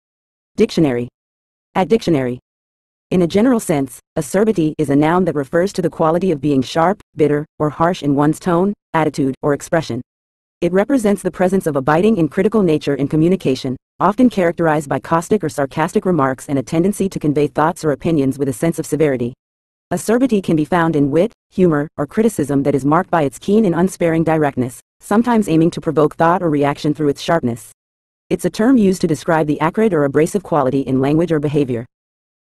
Acerbity Meaning In English
In a general sense, "acerbity" is a noun that refers to the quality of being sharp, bitter, or harsh in one's tone, attitude, or expression. It represents the presence of a biting and critical nature in communication, often characterized by caustic or sarcastic remarks and a tendency to convey thoughts or opinions with a sense of severity. Acerbity can be found in wit, humor, or criticism that is marked by its keen and unsparing directness, sometimes aiming to provoke thought or reaction through its sharpness. It's a term used to describe the acrid or abrasive quality in language or behavior.